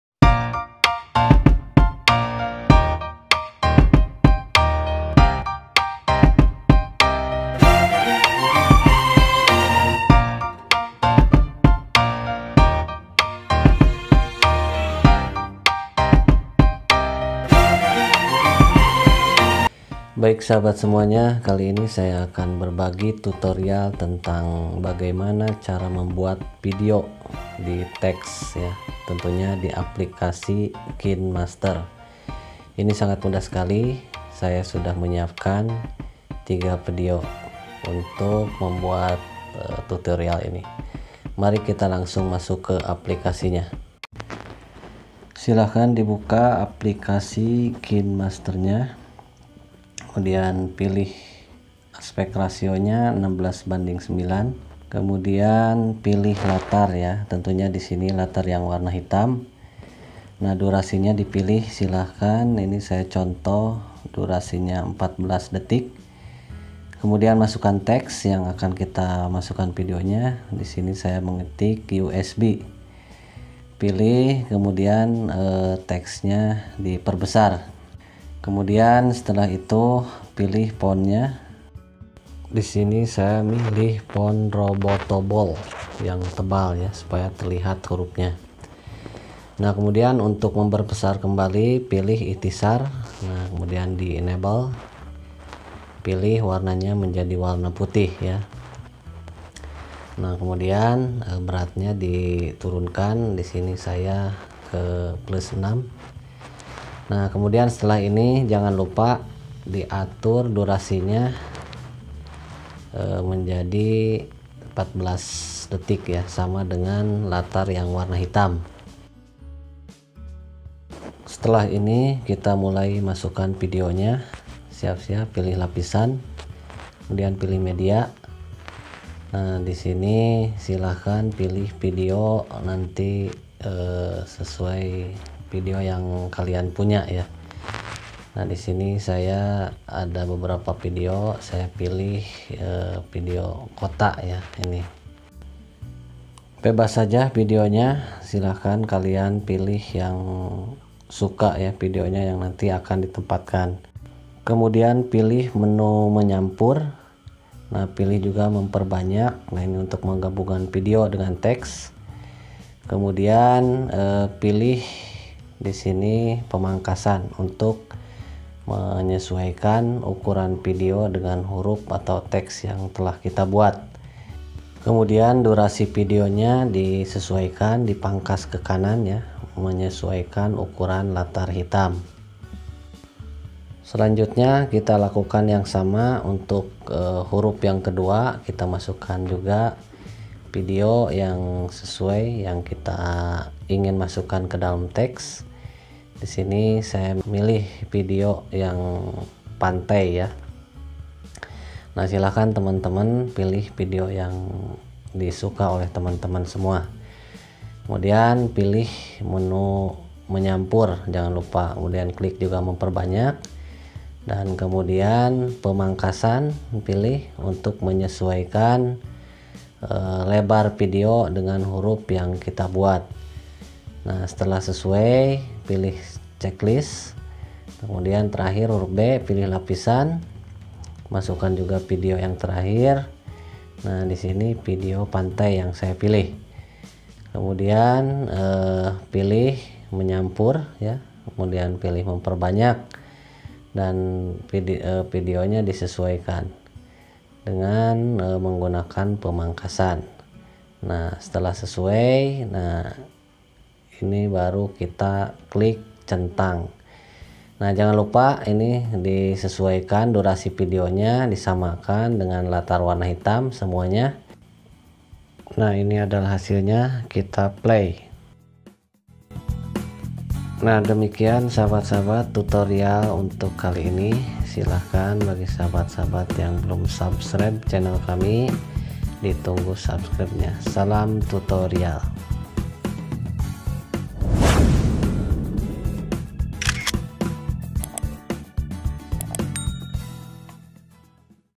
Cara Membuat VIDEO dalam TEKS di KINEMASTER Terbaru
Berikut adalah tutorial bagaimana membuat video berada di dalam teks dengan menggunakan aplikasi KINEMASTER, caranya sangat mudah sekali, tinggal menyiapkan beberapa video terlebih dahulu yang akan dimaasukan ke dalam teks